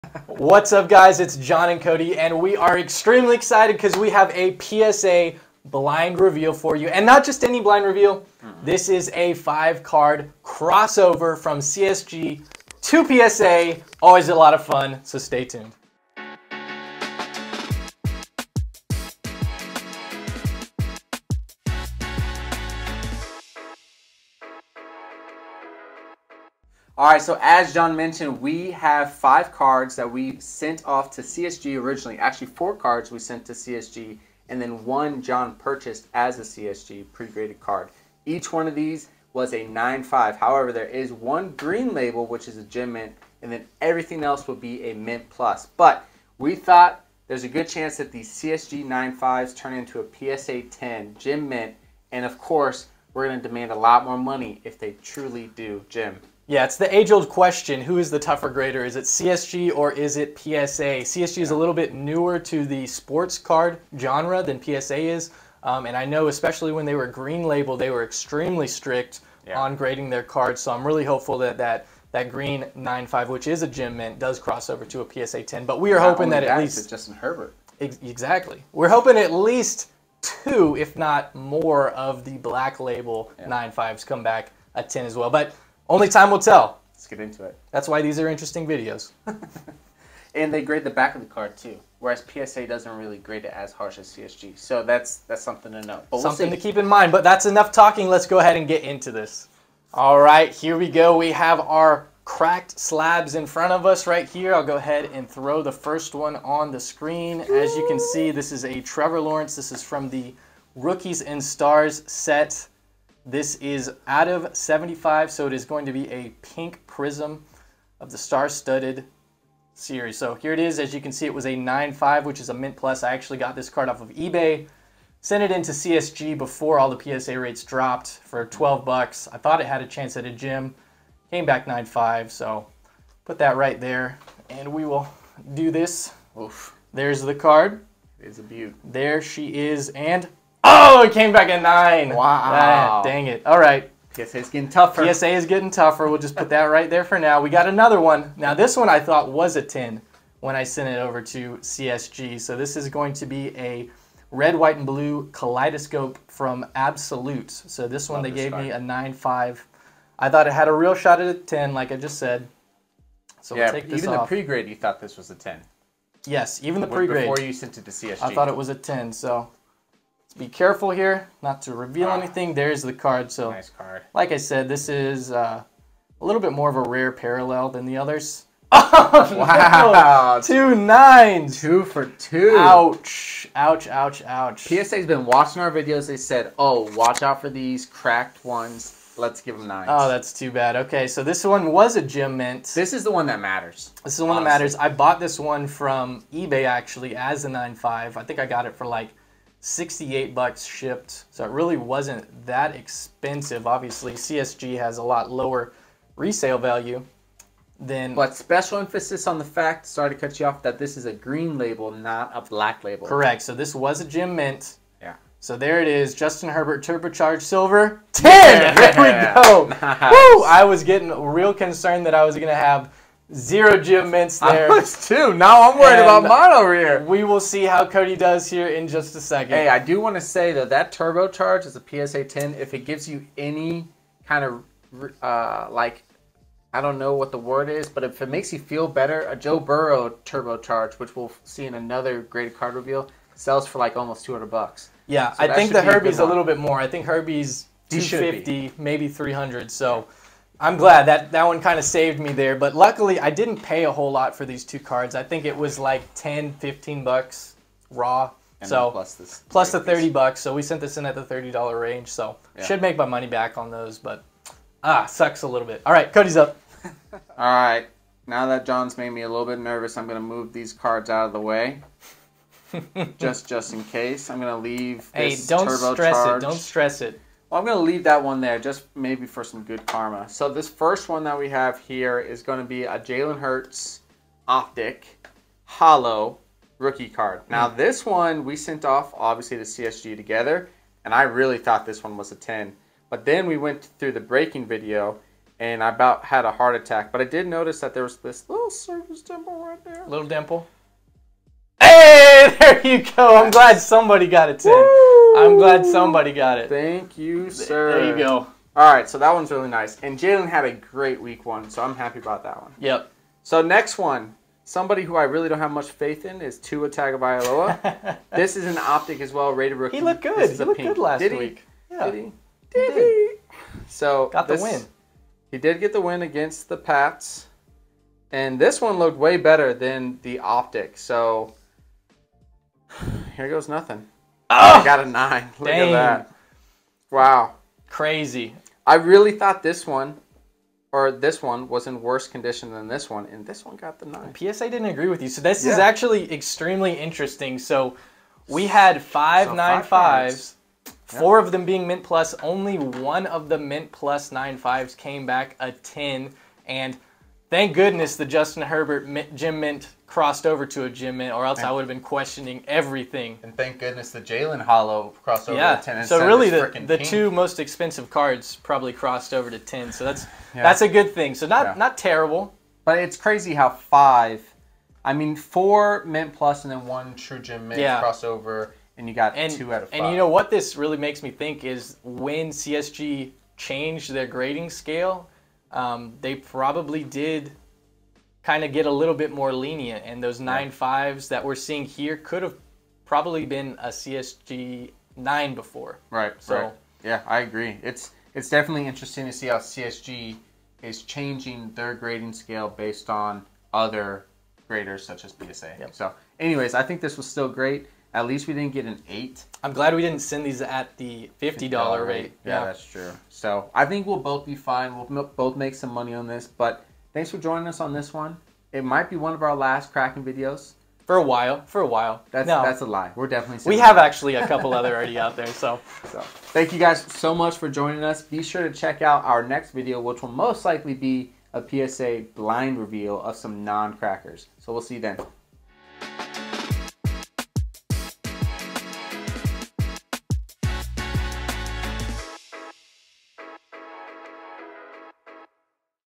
CSG to PSA Cracking Crossover! Does CSG 9.5 = PSA 10???
Who is the tougher grader, CSG or PSA? We cracked 5 CSG 9.5s and sent them to PSA to find out! Check out how we did and let us know your thoughts!